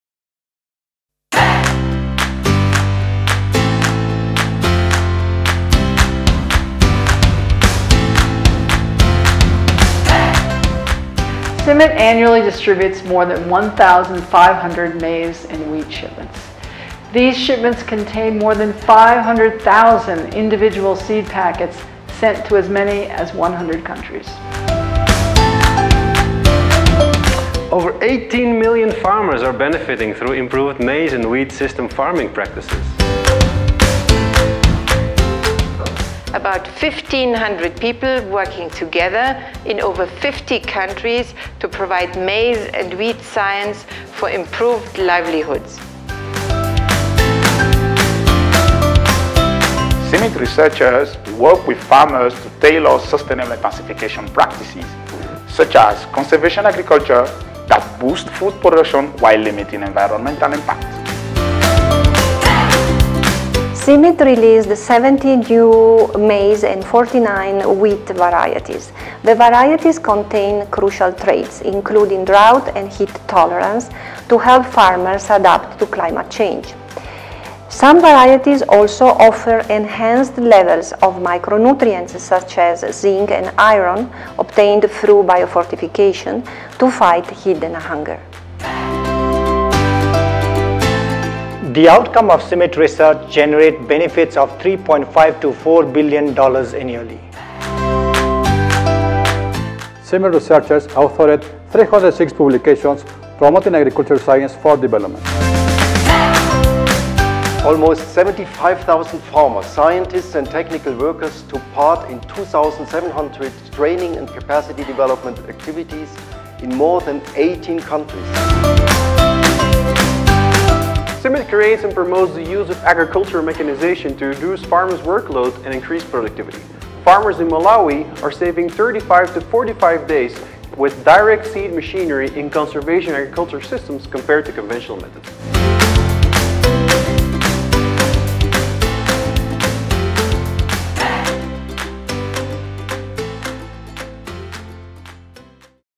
CIMMYT Annual report 2017: maize and wheat for improved livelihoods
The report details CIMMYT and its partners’ role in responding to several global challenges to maize and wheat agriculture, including wheat blast in Bangladesh, fall armyworm in Africa and the need to replenish maize seed supplies in Haiti. In 2017, CIMMYT strengthened its presence in China through a new research center established at Henan Agriculture University and forged new partnerships with companies in Mexico to support the responsible sourcing of maize and wheat grain. This work is made possible through collaborations in over 70 countries. to download different versions.